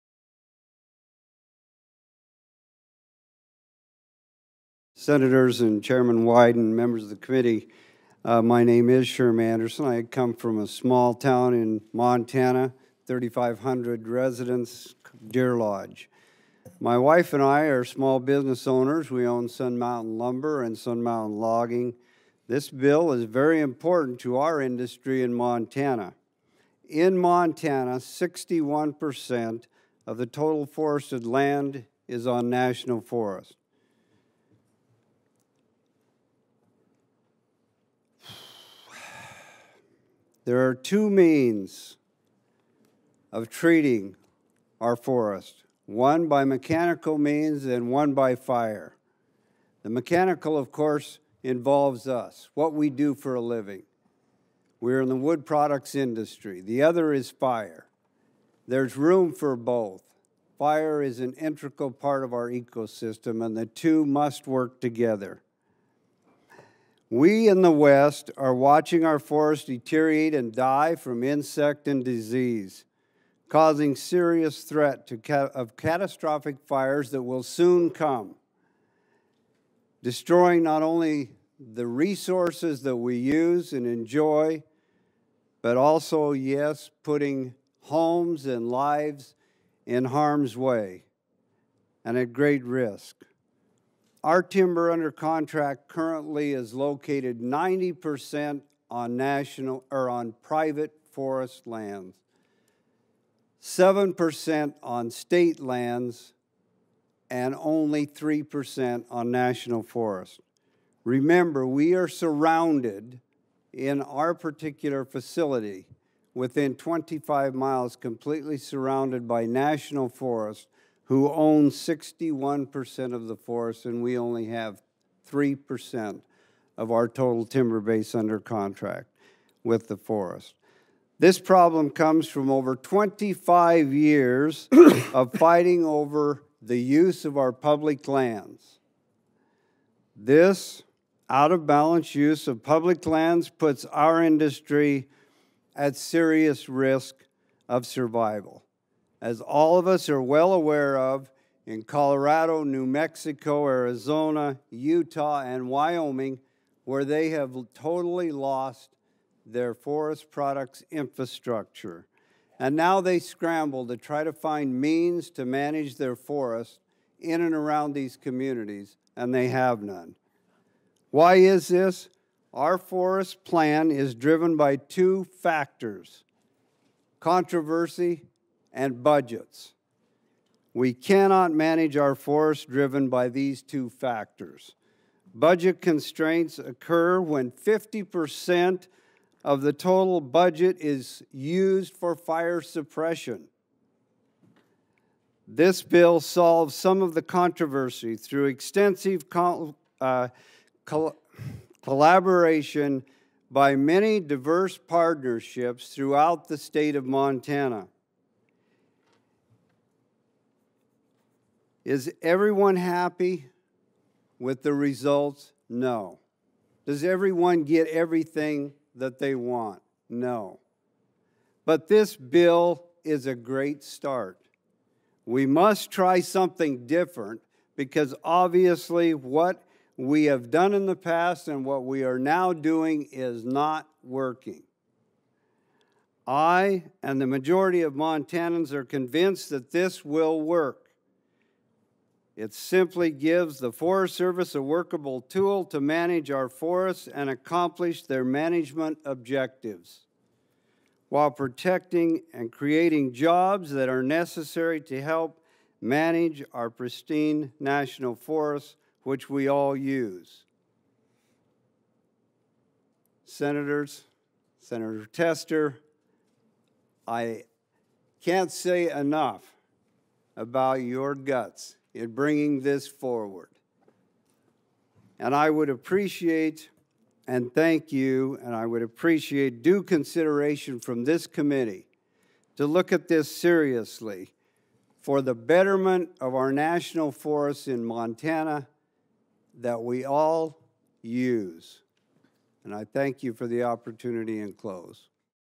Forest Jobs and Recreation Act Hearing: Sherm Anderson
Sherm Anderson, owner of Sun Mountain Lumber in Deer Lodge, MT, testifies before the Senate Public Lands and Forests Subcommittee on the Forest Jobs and Recreation Act